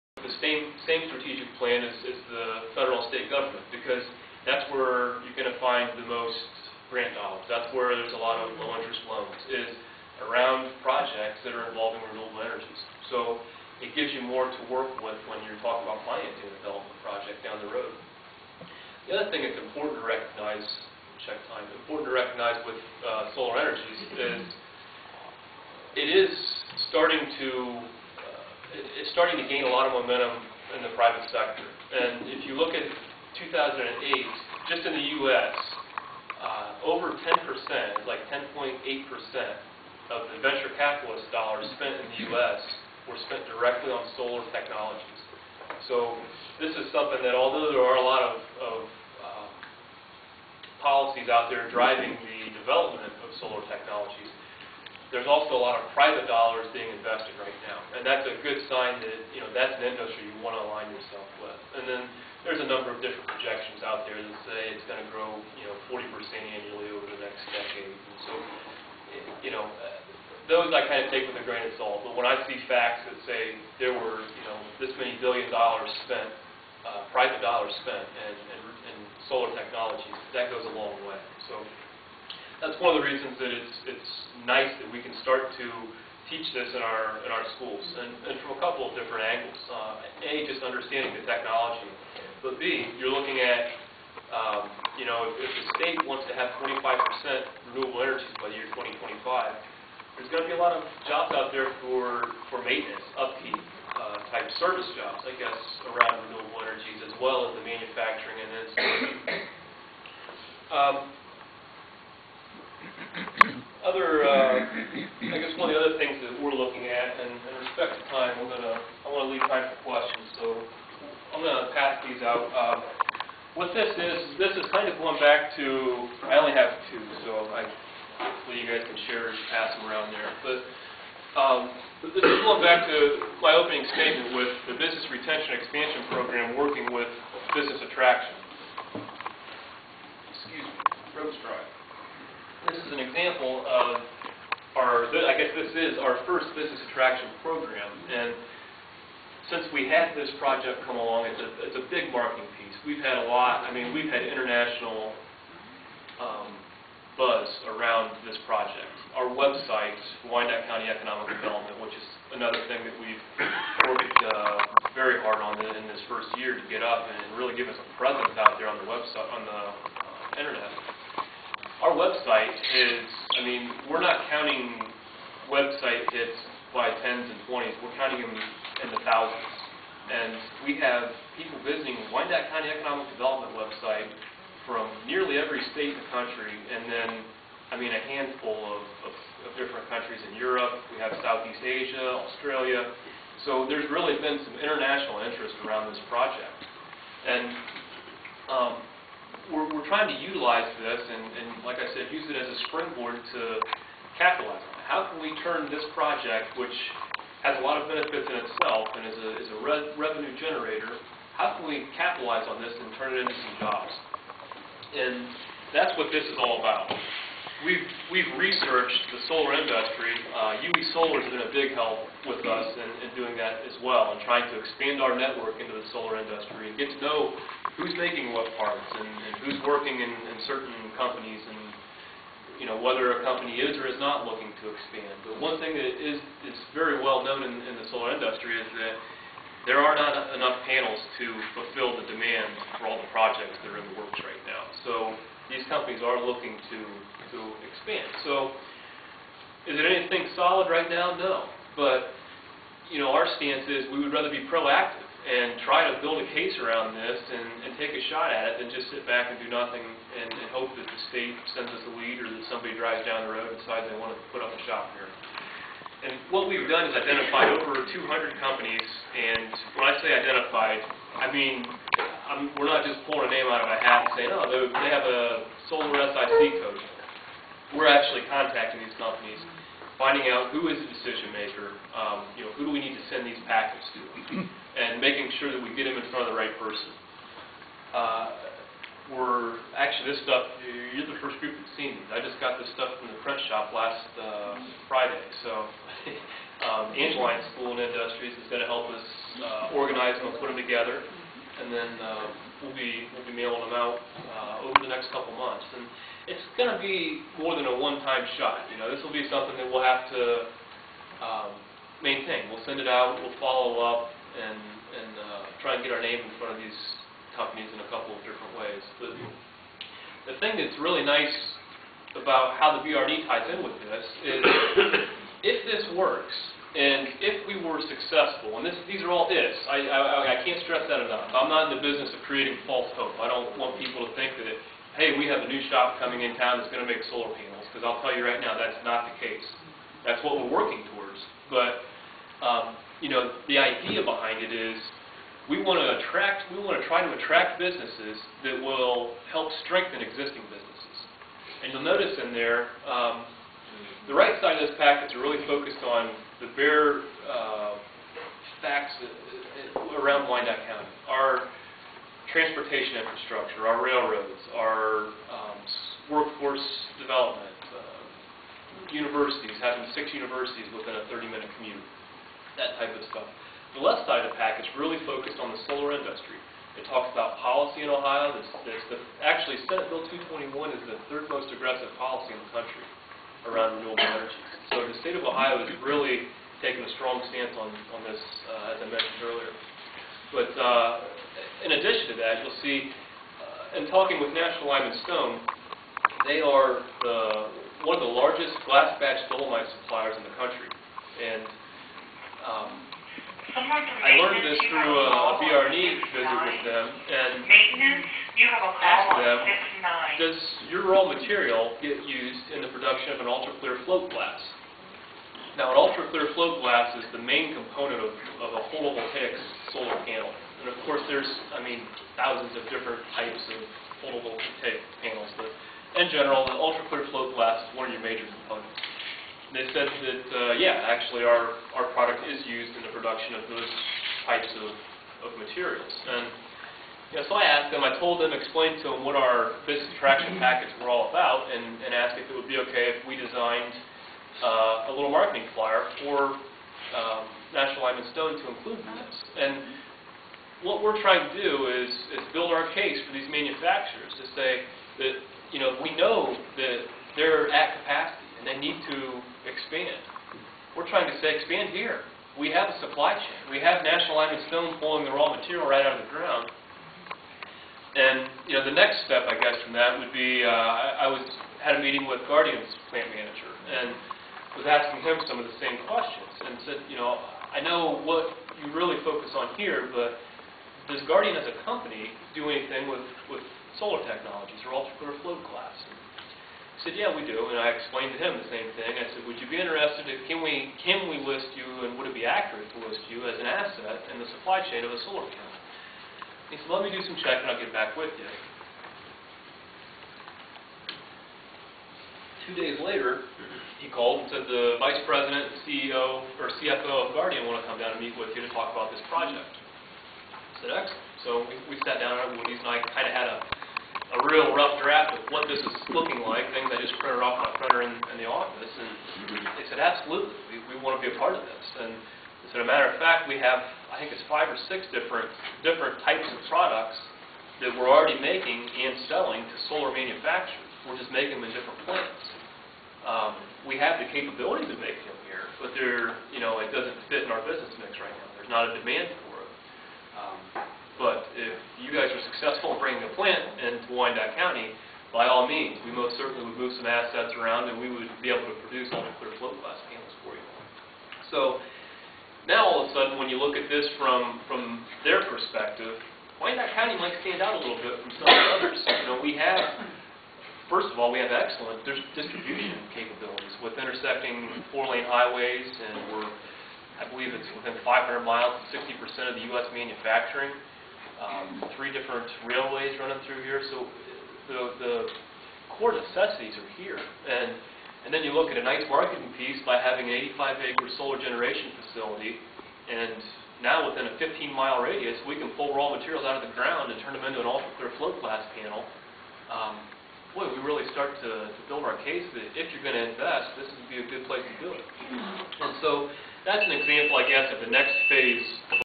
Wyandot County In Persuit Of Solar Energie
Wyandot County Director for Economic Development Eric Romich highlights the benefits and importance of solar energy at fairhaven Community's monthly breakfast meeting.